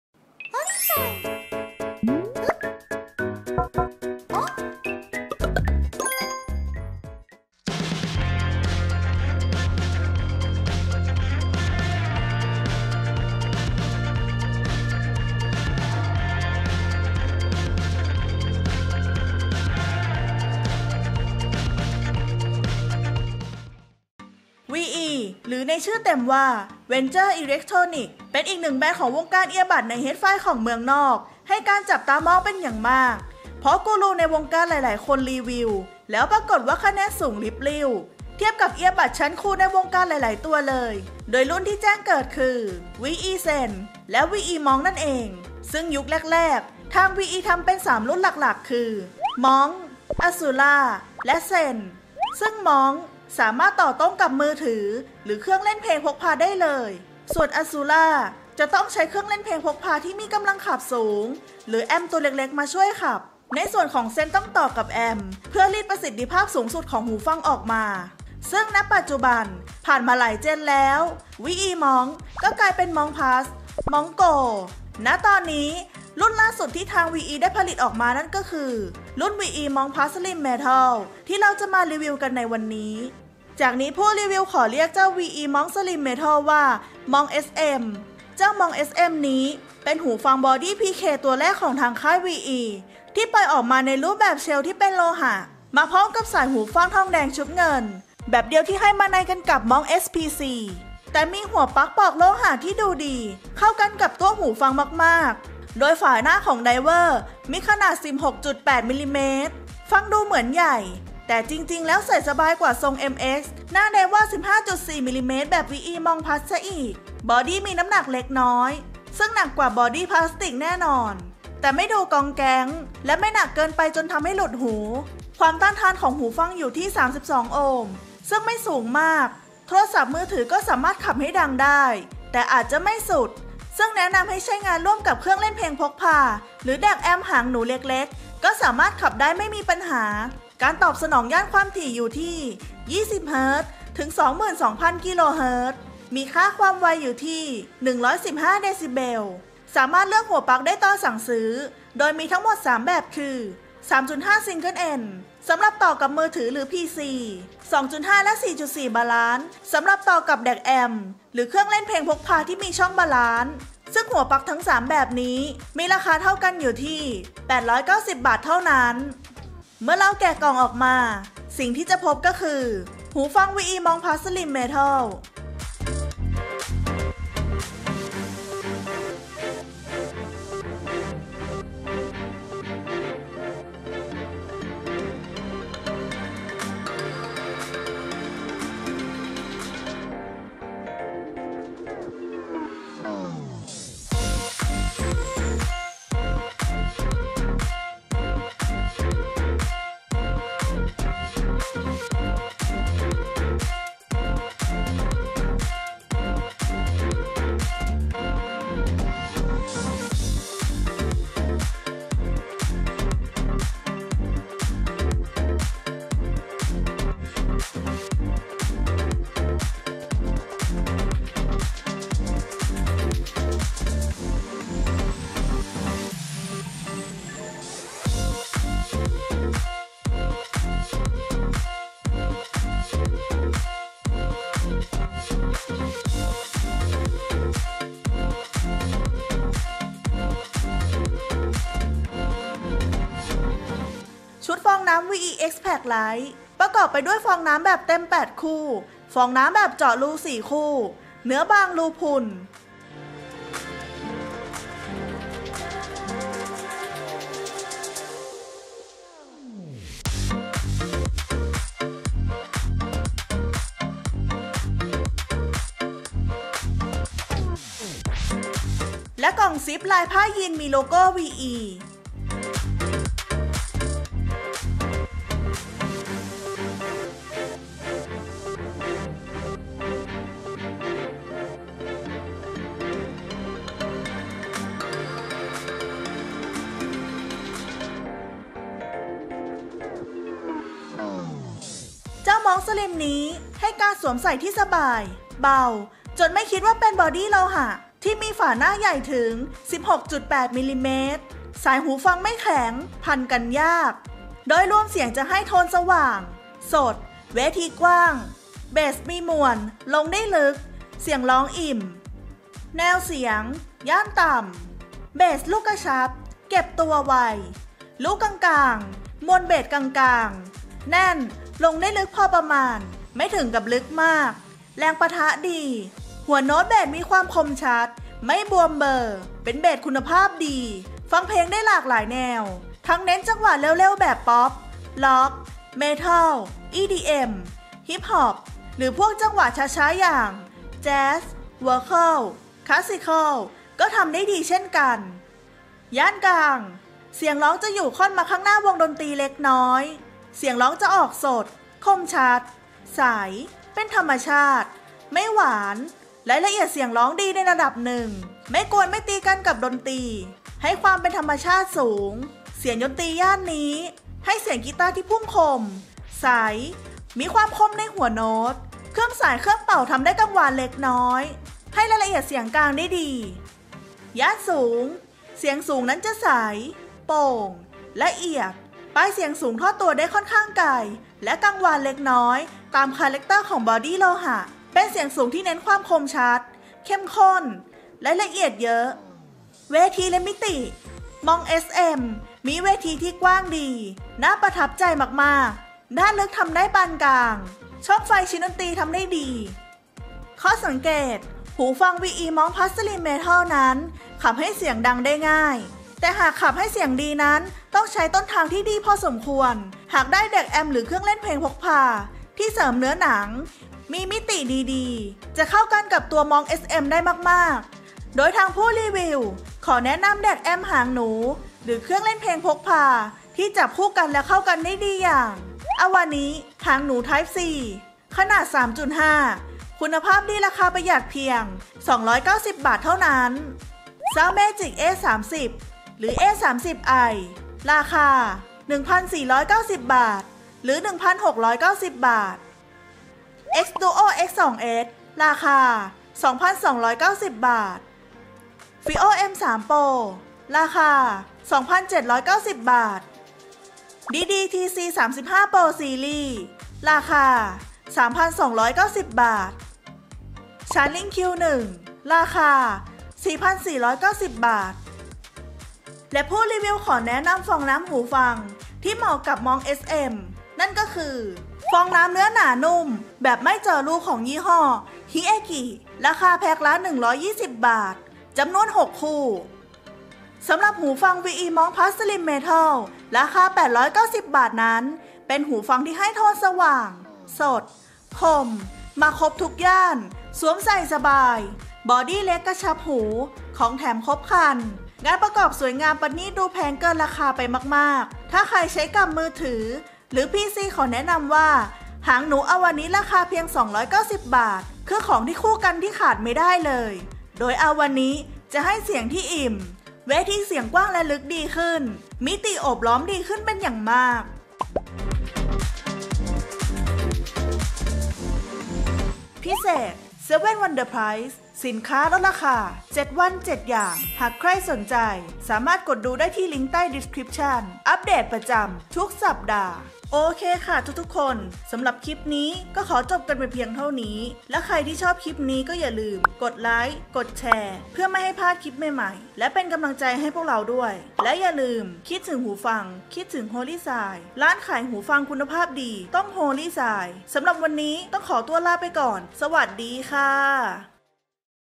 Holysai Review : VE MONK SLIM METAL หูฟังเอียบัดบอดี้ PK บอดี้โลหะ ตัวแรกของค่าย VE ที่คุ้นเคย!◄027►
ดูรายละเอียด VE MONK SLIM METAL เพิ่มเติมได้ที่
ส่งเก็บเงินปลายทางฟรี , รูดบัตรปลายทางฟรี , มีบริการส่งด่วนผ่าน LALAMOVE